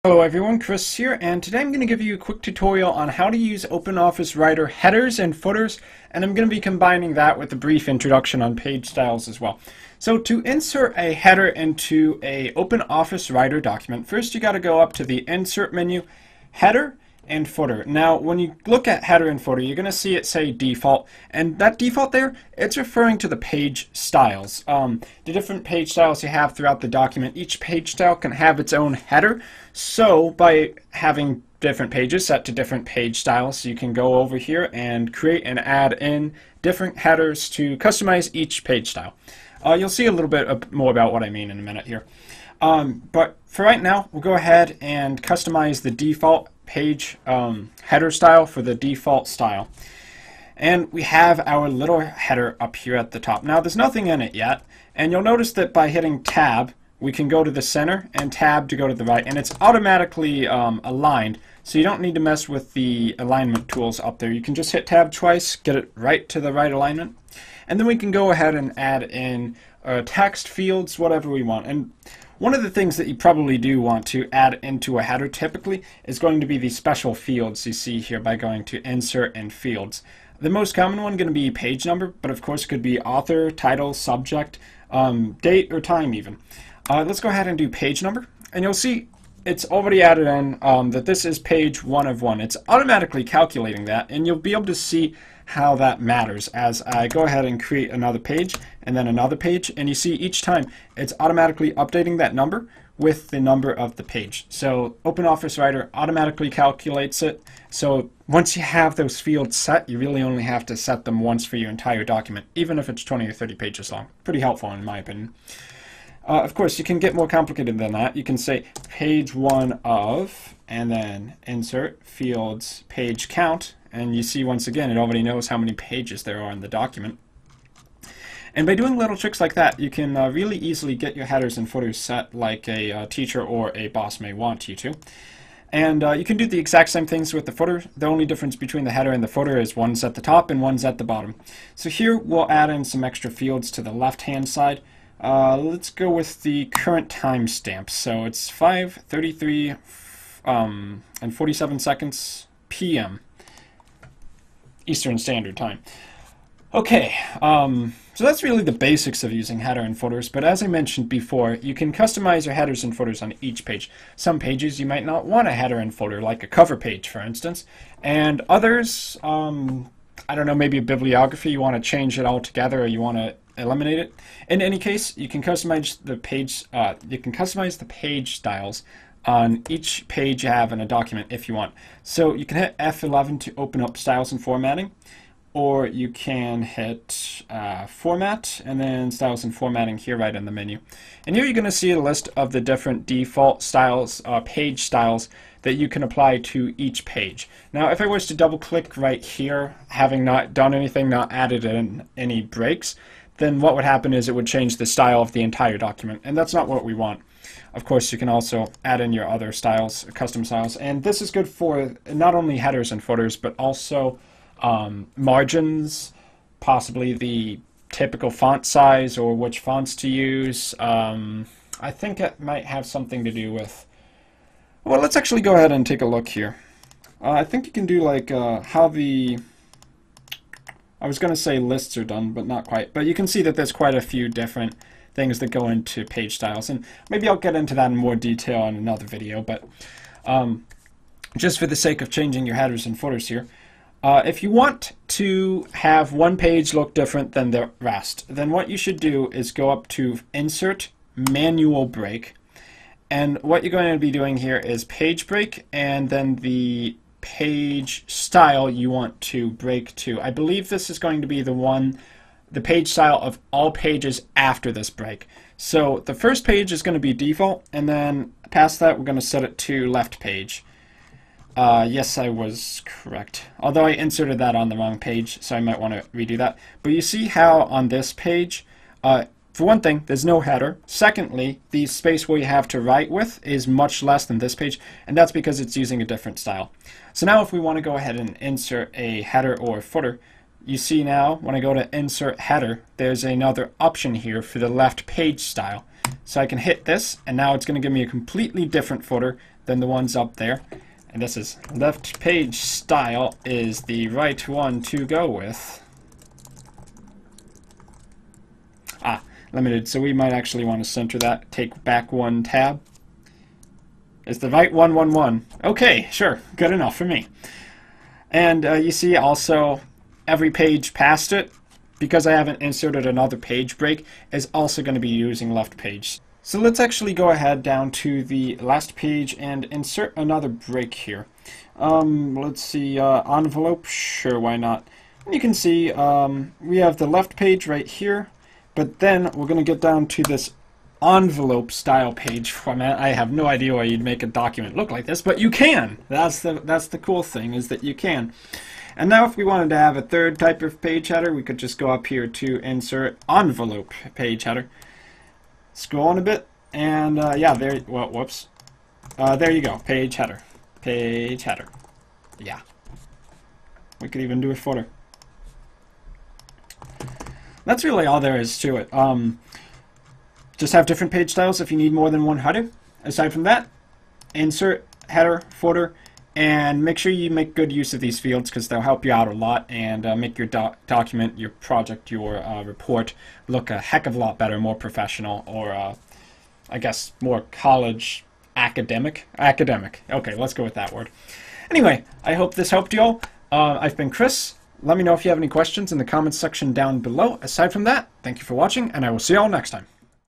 How to Insert Headers and Footers in Open Office - One or Multiple Page Styles
This video will show you how to insert headers and footers within Open Office Writer. These go at the top and bottom of your pages and can carry information over from page to page.